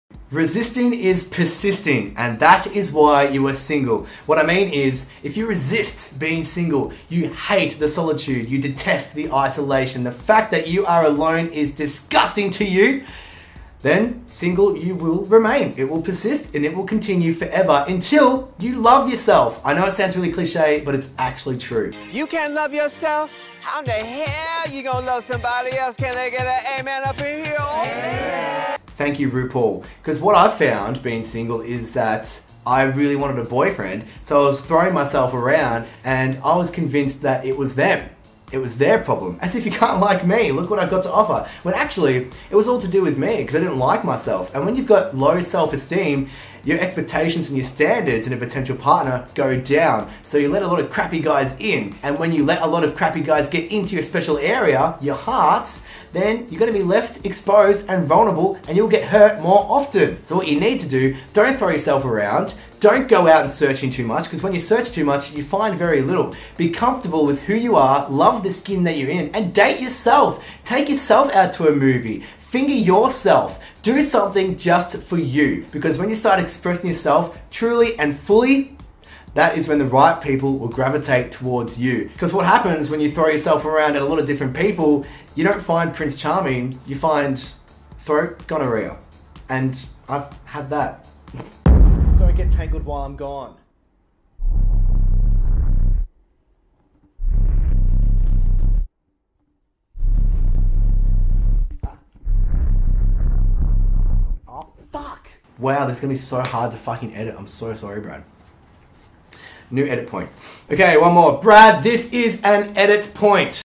this is why you're single
I rant about why you're single and how to stop being single